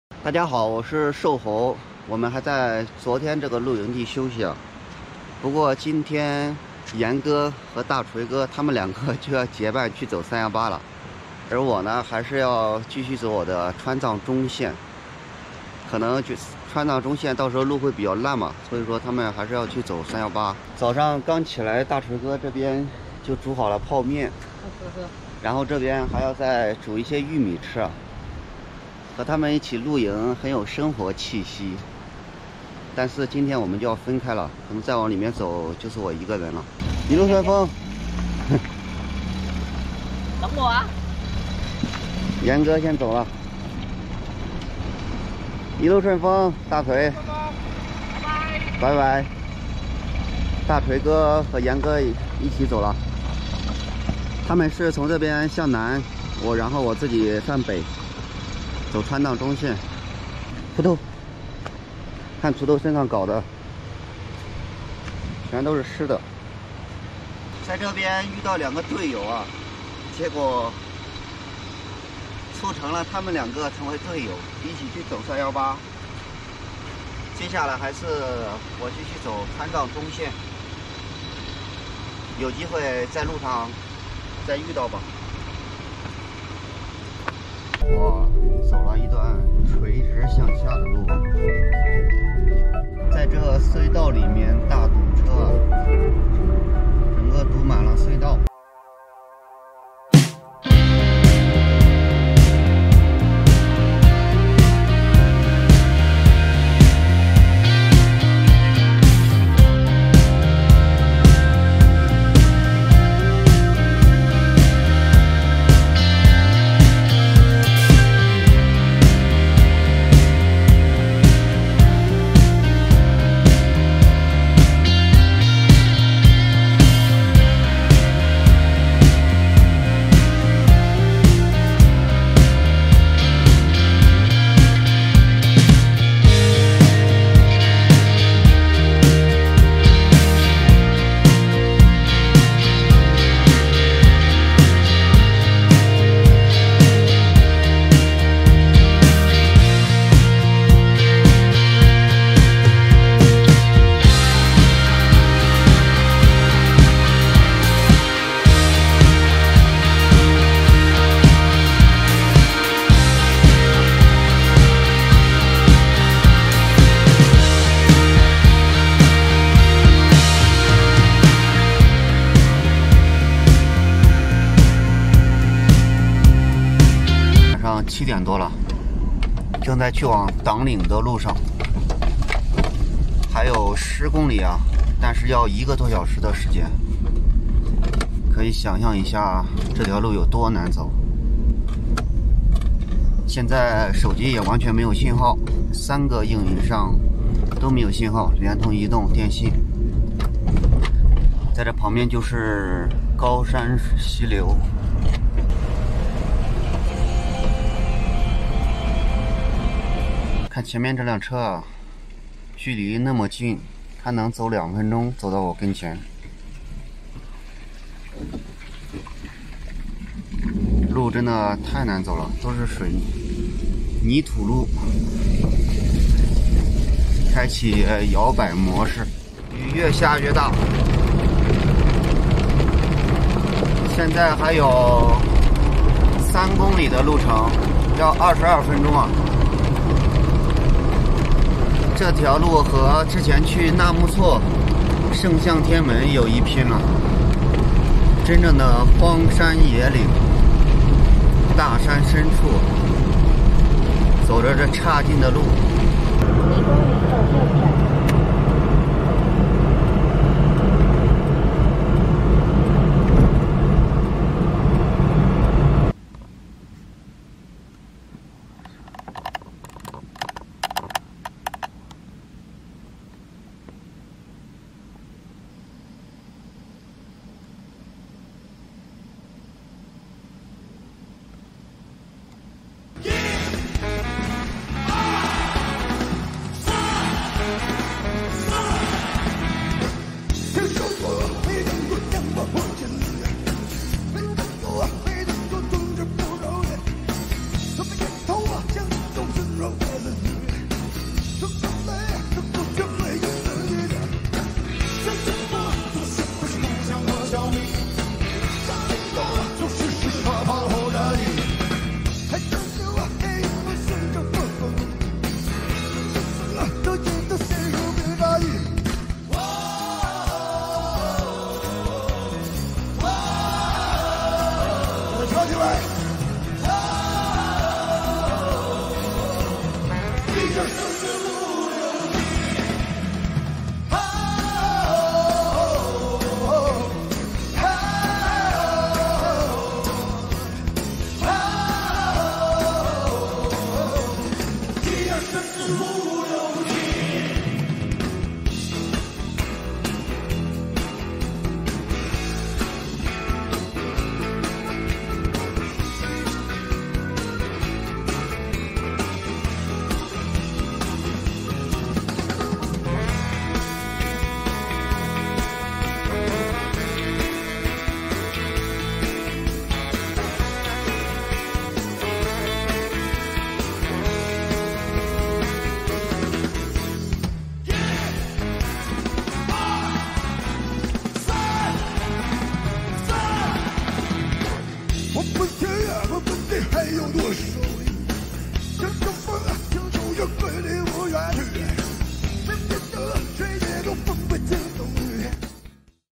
和隊友們分開後，獨自走過沒有信號的崎嶇爛路邂逅絕美風景，一人一狗繼續向著西藏出發 | 用青春去旅行
和隊友們分開後，獨自走過沒有信號的崎嶇爛路竟邂逅絕美風景，一人一狗繼續向著西藏出發 | 用青春去旅行

隊友們打算走318航線，我還是繼續走川藏中線，和他們分開後又獨自上路了，一人一狗繼續向著西藏出發。在走過一段崎嶇的山路後，竟然遇到了一段美妙的風景。
麵包車窮游中國，分享沿途美好！

我這邊盡力追上更新進度，希望大家多多支持，打擊盜版，謝謝！